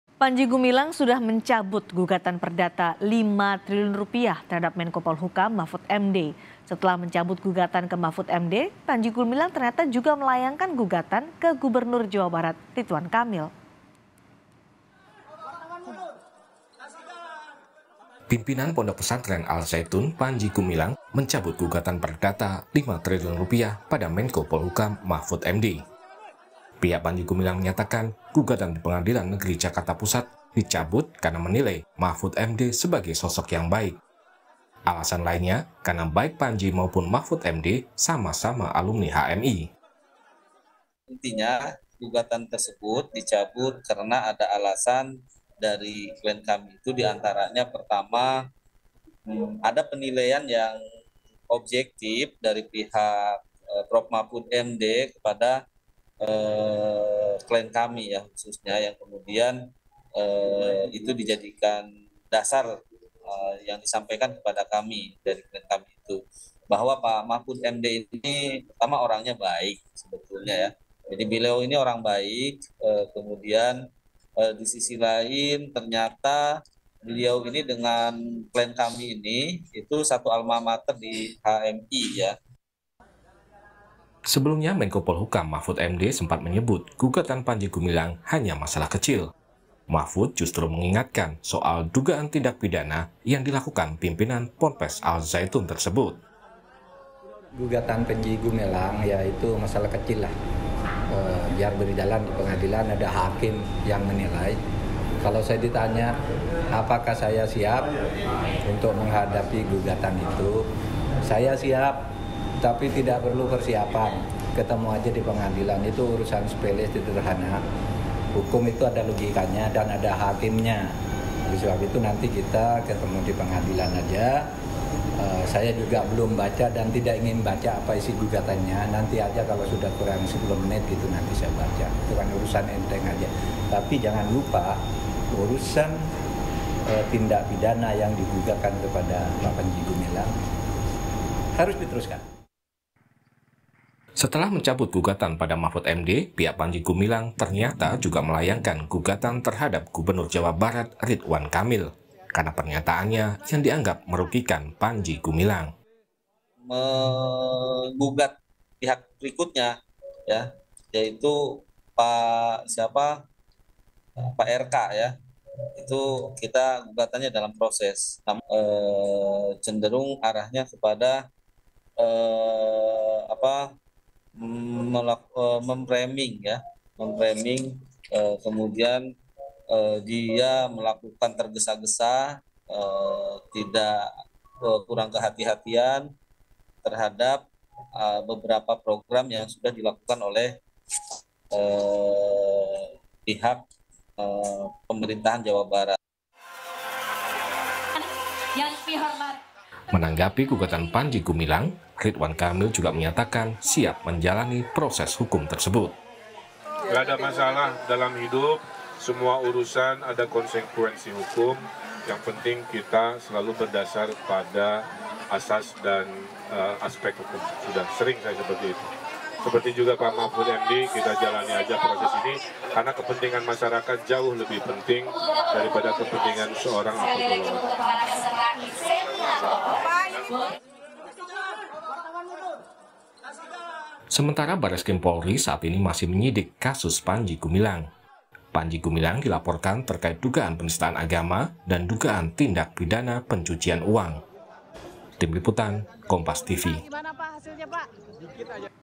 Cabut Gugatan pada Mahfud MD, Kini Panji Gumilang Gugat Ridwan Kamil
KOMPAS.TV - Panji Gumilang telah mencabut gugatan perdata Rp5 triliun terhadap Menko Polhukam Mahfud MD.

Setelah mencabut gugatan ke Mahfud MD pihak Panji Gumilang ternyata juga melayangkan gugatan ke Gubernur Jawa Barat Ridwan Kamil.

Sebelumnya, Ridwan Kamil menegaskan apa yang disampaikannya terkait kasus yang menyeret Panji Gumilang sudah sesuai prosedur.

RK menyebut, pihaknya siap menerima konsekuensi atas tindakan dan pernyataannya itu lantaran untuk kepentingan masyarakat.

Sementara, Bareskrim Polri saat ini masih menyidik kasus Panji Gumilang. Panji Gumilang dilaporkan terkait dugaan penistaan agama dan dugaan pindak pidana pencucuian uang.

Baca Juga Tersangka di Kasus Panji Gumilang Belum Ditetapkan, Kapolri: Butuh Kecermatan di